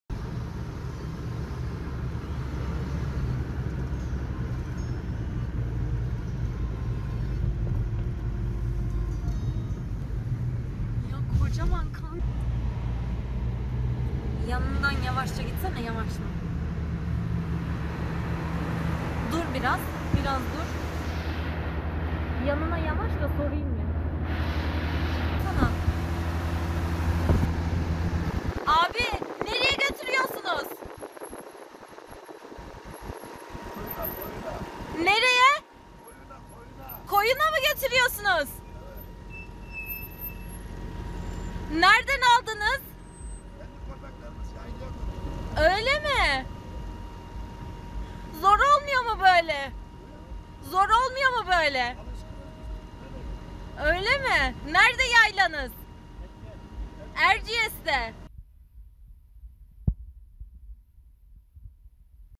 Trafikte gözden kayboldu...Gören vatandaşlar şaşkınlıklarını gizleyemedi!
Kayseri'de çekilen görüntülerde, bir otomobilin arka koltuğu ve bagajında iki büyük kangal köpeğini görenler şaşkınlıklarını gizleyemedi.